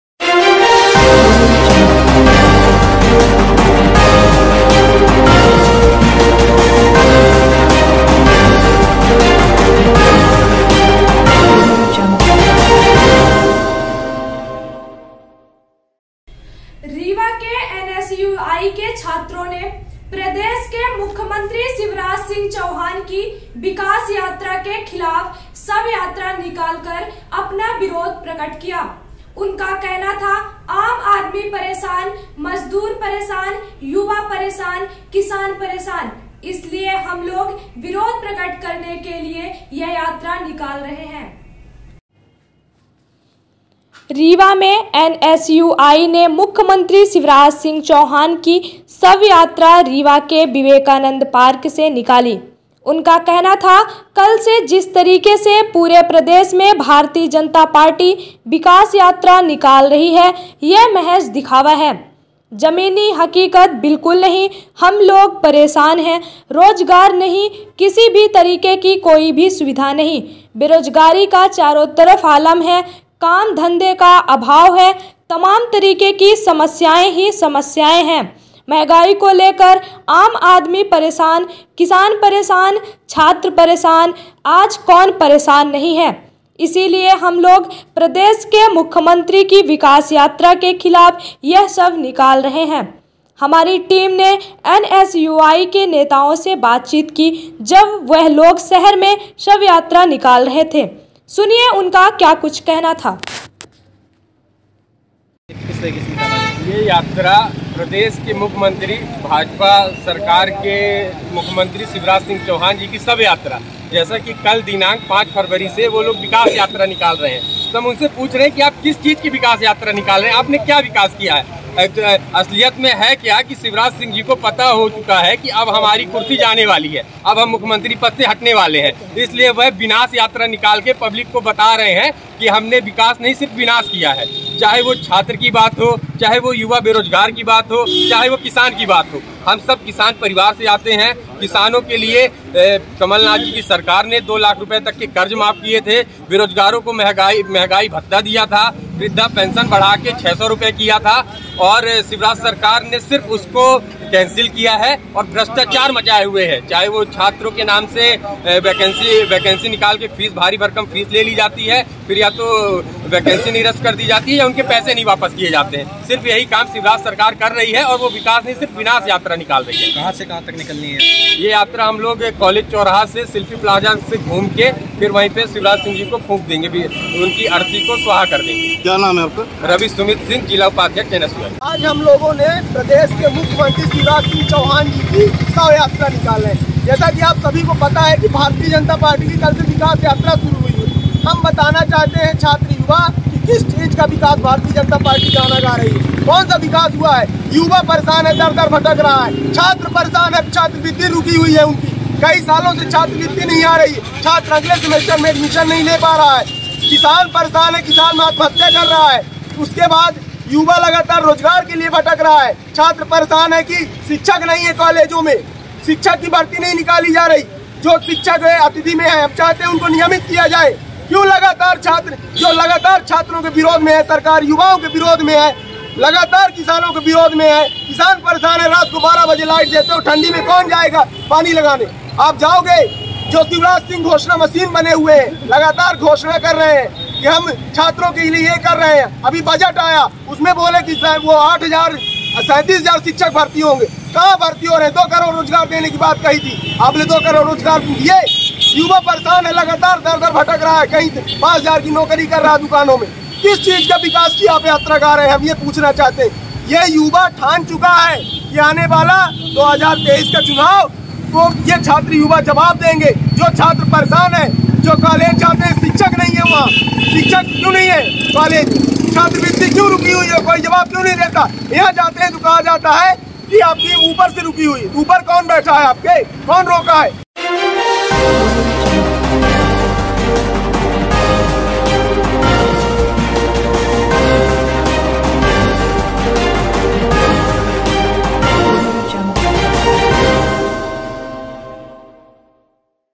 एनएसयूआई ने विकास यात्रा के खिलाफ विवेकानंद पार्क से शव यात्रा निकालकर विरोध दर्ज कराया ||NNT
एनएसयूआई ने विकास यात्रा के खिलाफ विवेकानंद पार्क से  शव यात्रा निकालकर विरोध दर्ज कराया